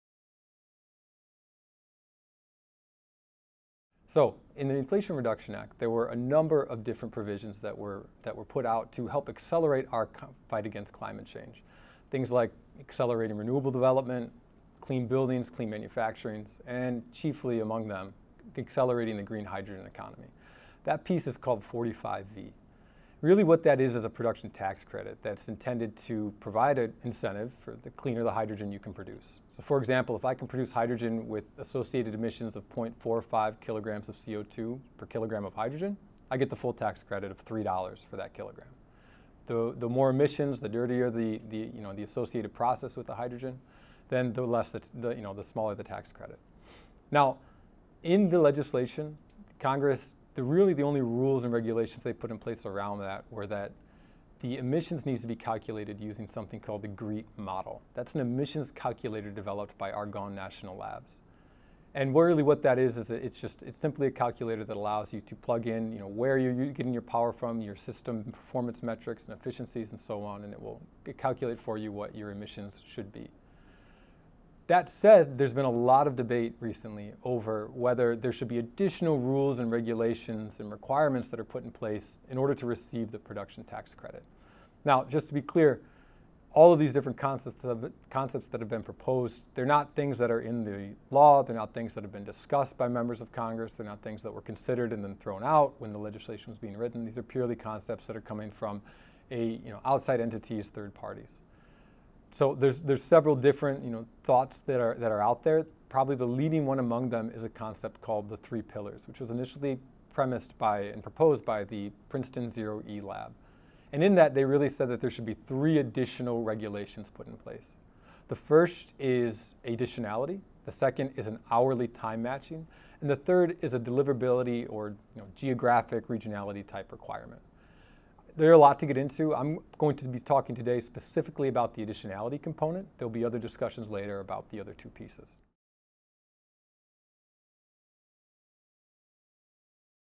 Plug’s Luke Wentlent discusses green hydrogen components in the landmark Inflation Reduction Act
Plug’s Luke Wentlent discusses what’s included in the landmark Inflation Reduction Act designed to accelerate the buildout of the green hydrogen economy